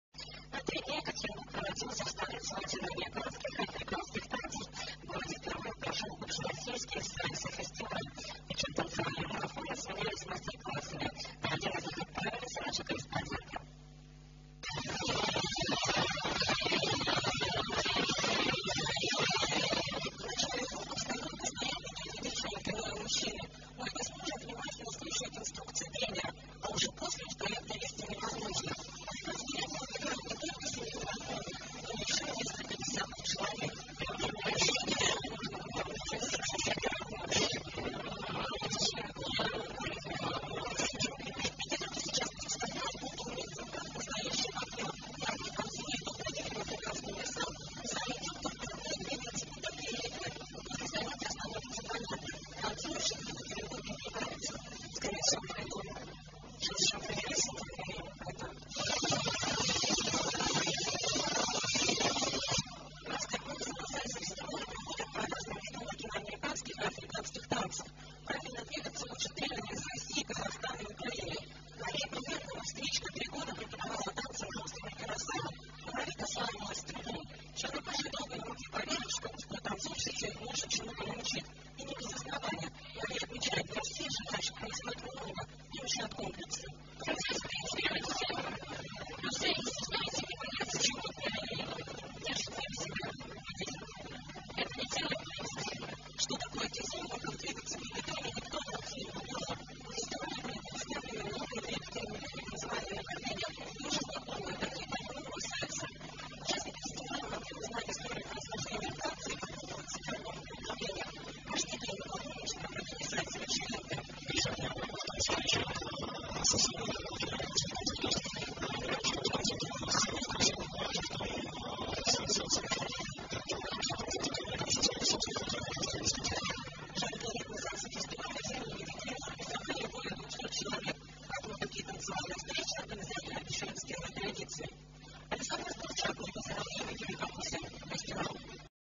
Mambo Tribe Fest in Yekaterinburg @ Vesti-Ural TV Channel
Mambo Tribe Fest in Yekaterinburg @ Vesti-Ural TV Channel | Мамбо Трайб Фест — Екатеринбург в эфире ТВ-канала «Вести-Урал»
20—22 January 2012 | 20—22 января 2012 года
Yekaterinburg, Russia | Екатеринбург, Россия